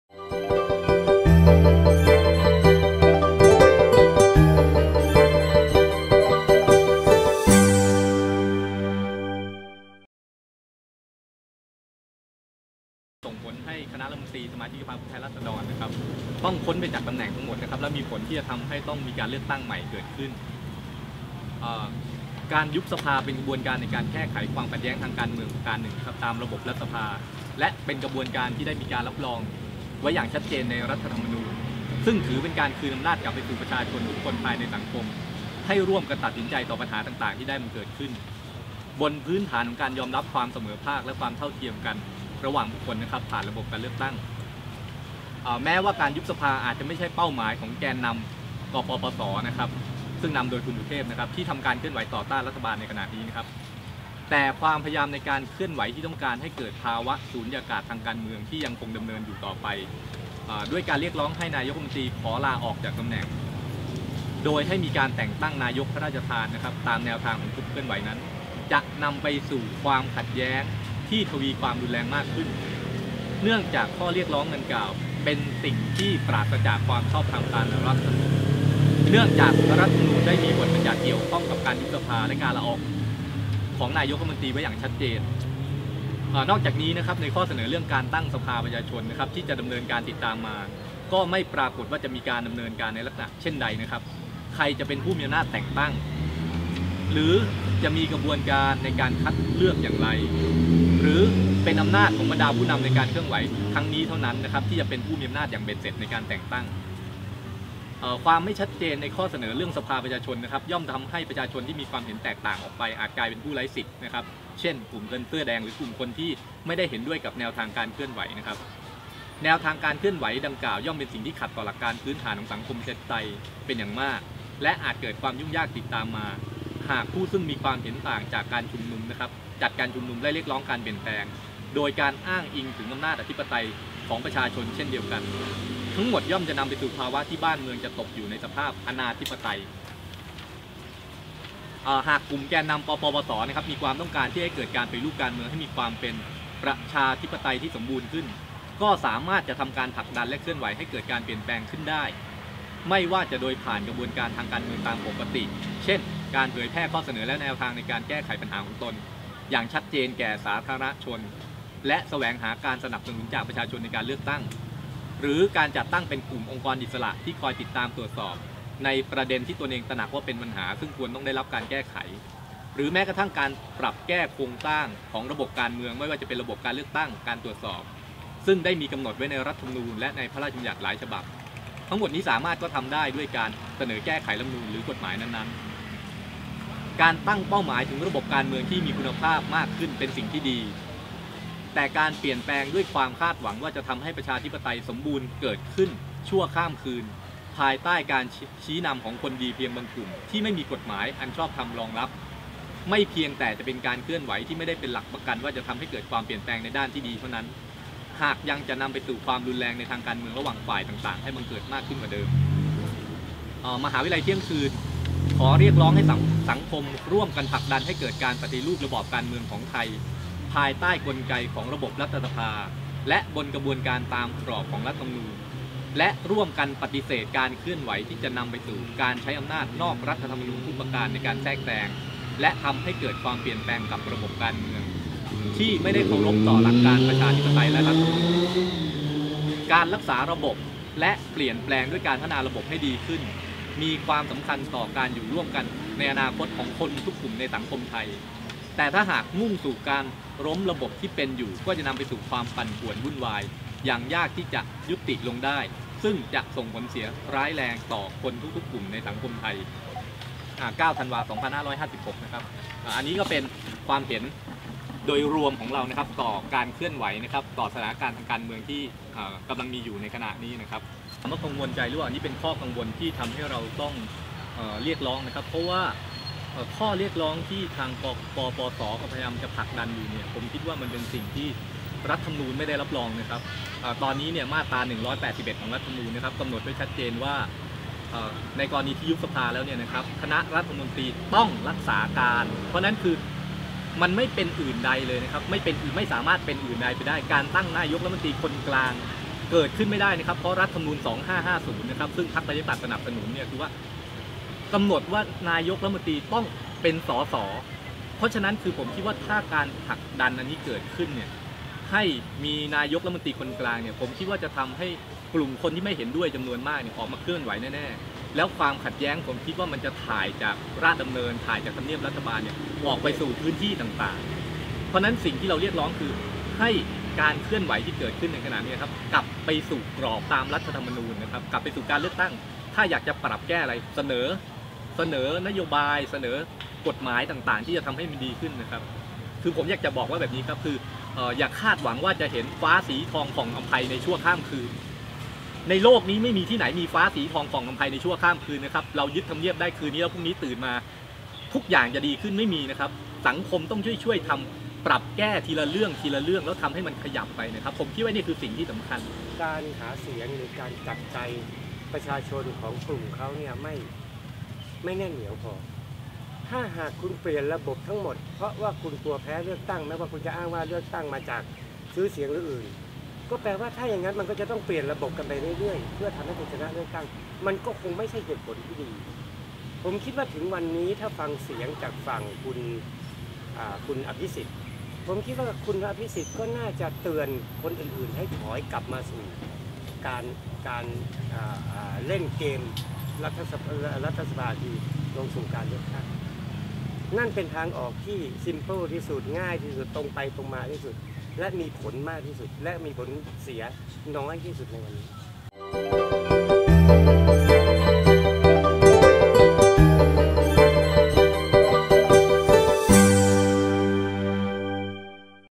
แถลงการณ์มหาวิทยาลัยเที่ยงคืนหยุดการเคลื่อนไหวที่ทำลายระบบประชาธิปไตย
แถลงการณ์มหาวิทยาลัยเที่ยงคืน "หยุดการเคลื่อนไหวที่ทำลายระบบประชาธิปไตย" และตอบคำถามผู้สื่อข่าว